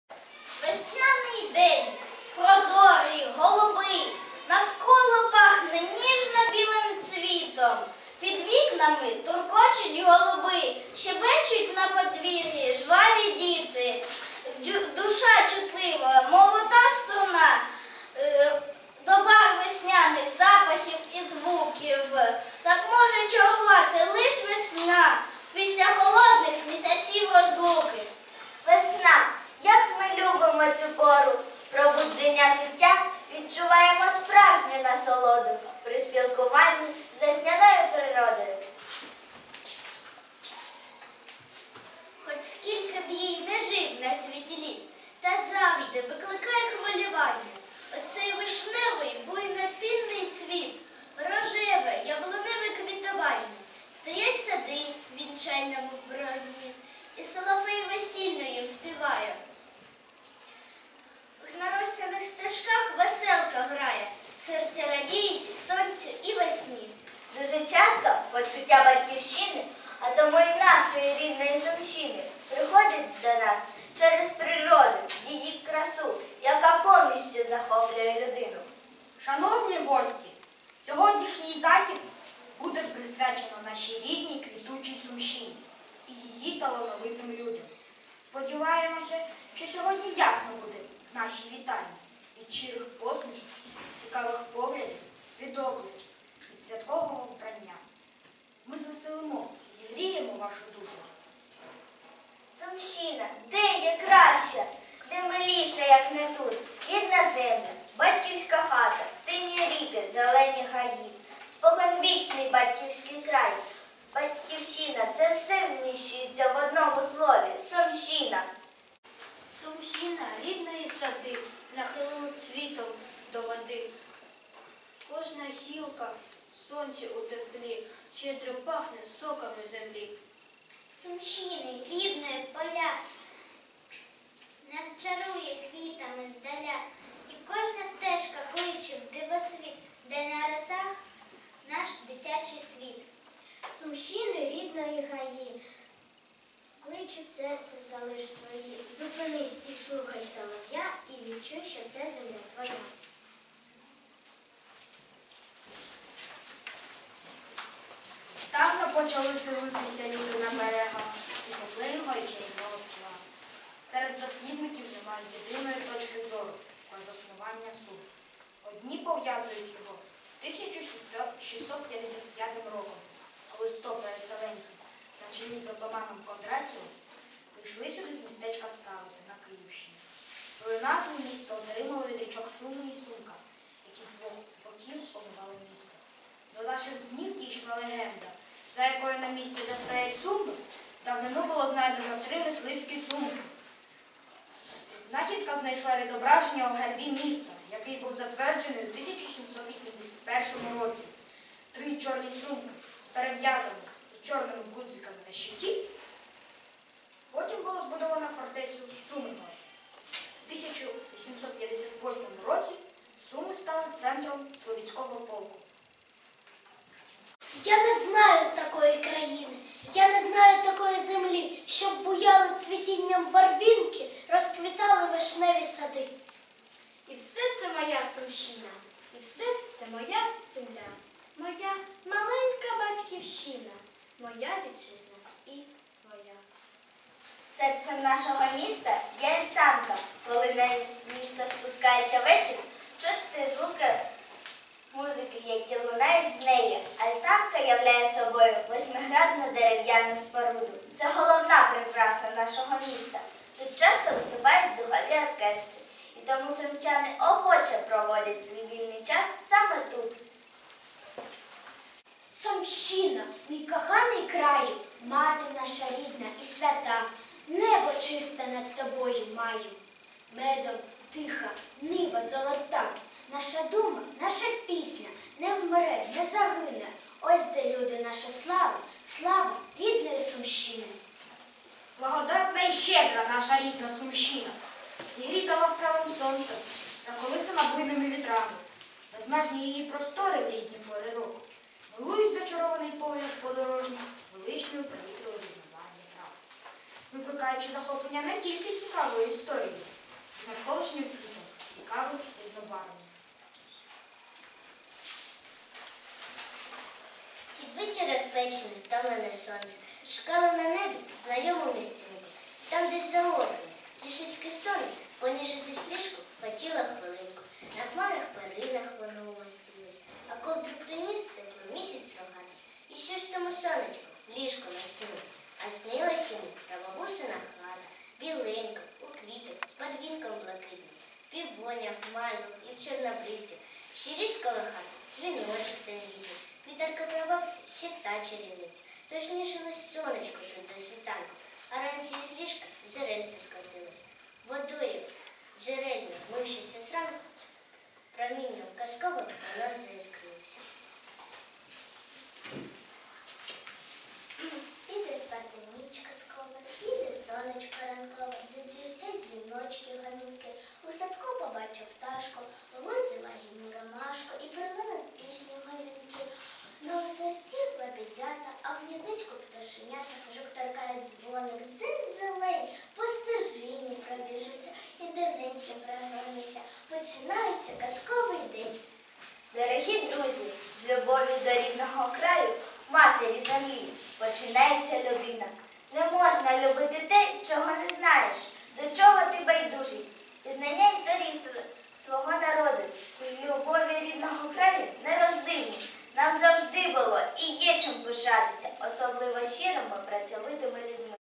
Літературно-поетична композиція "Рідне місто над Пслом" в 3-Б класі СШ №10 м. Суми
Літературно-поетична композиція "Рідне місто над Пслом" в 3-Б класі СШ №10 м. Суми 20 березня 2017 року.